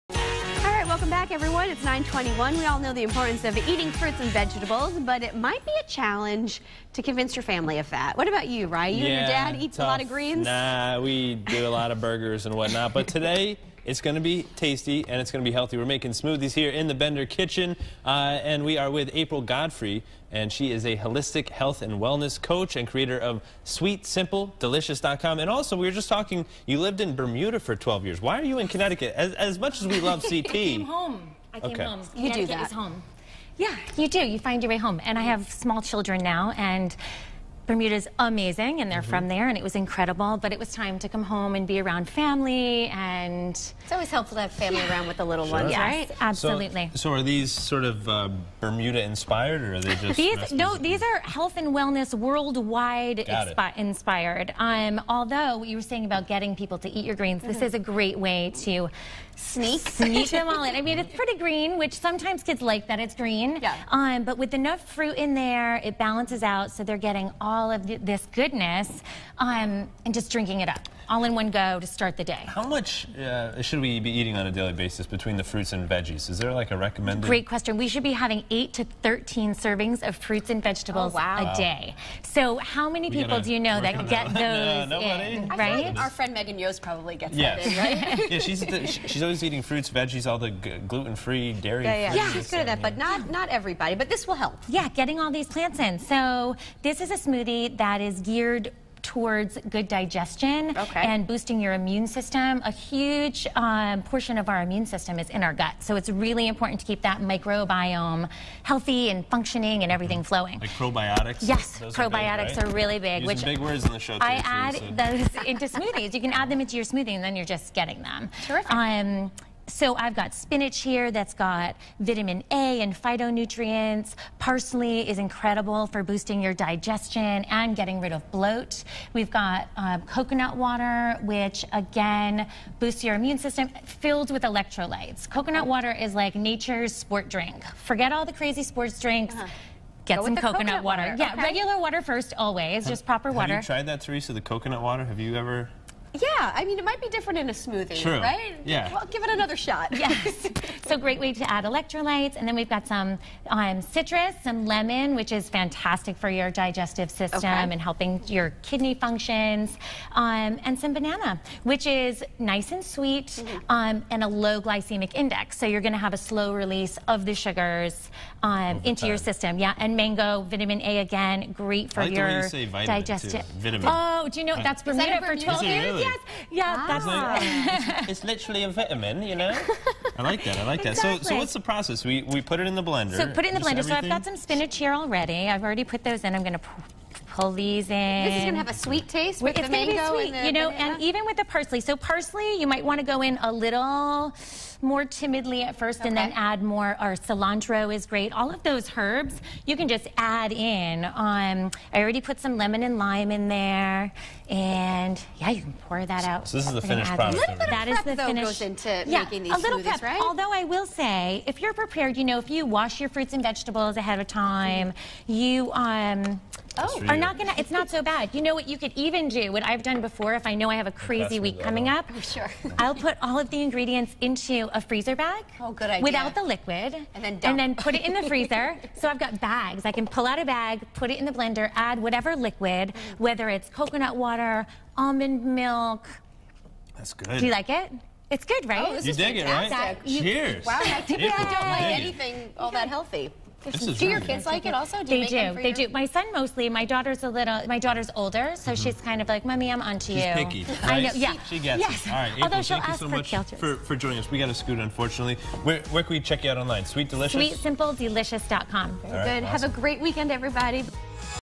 In the Bender Kitchen: Delicious and Nutritious Smoothies
Smoothies are a great way to make sure that your children are getting enough fruits and vegetables.